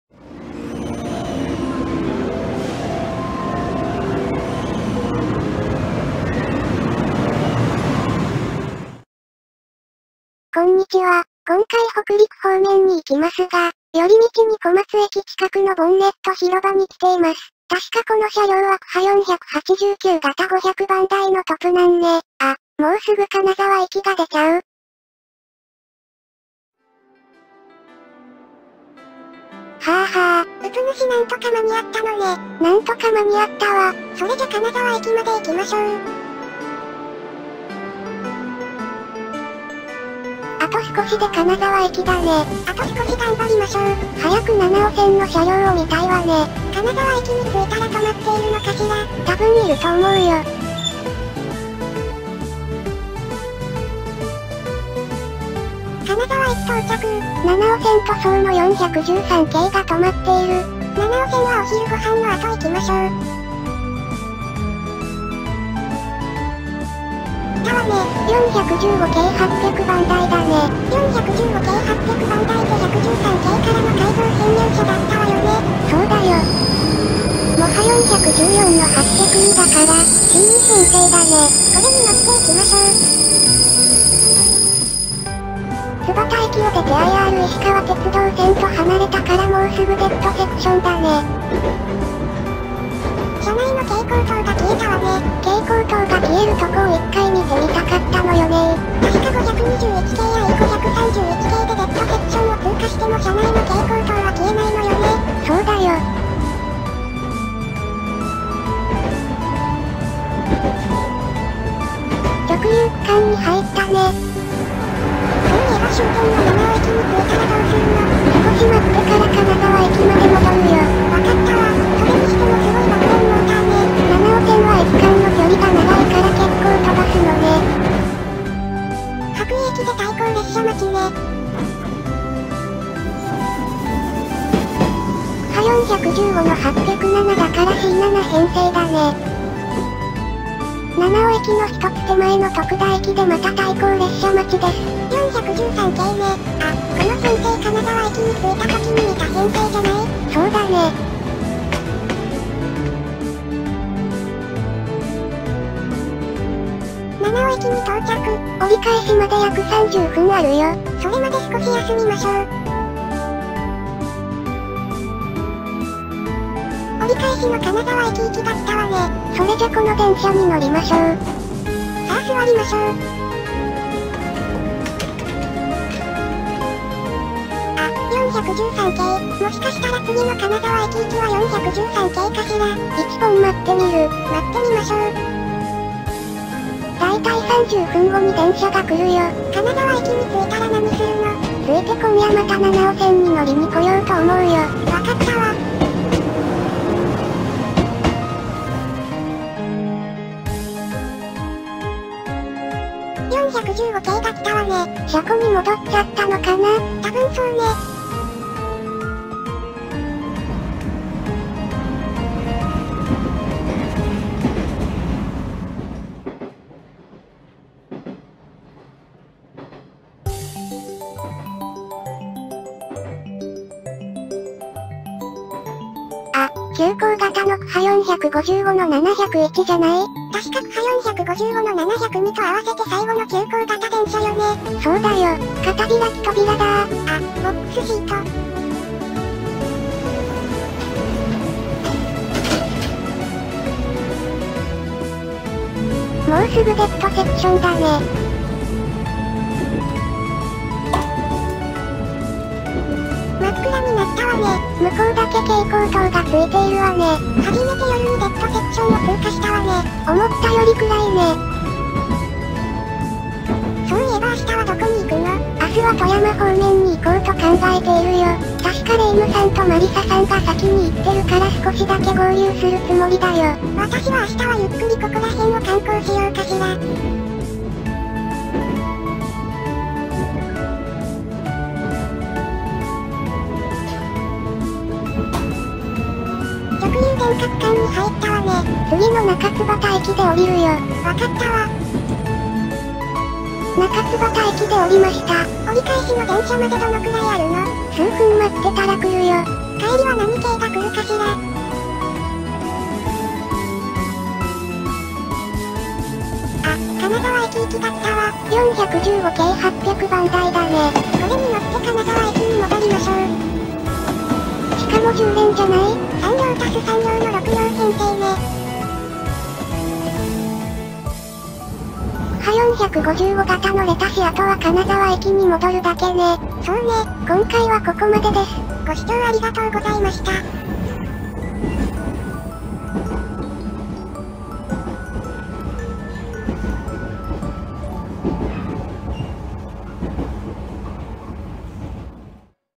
【鉄道旅ゆっくり実況】第4回 北陸へ！part1
青春18きっぷで北陸方面に行ったので。

※撮影日2020年8月（調整に時間がかかって投稿が遅れました）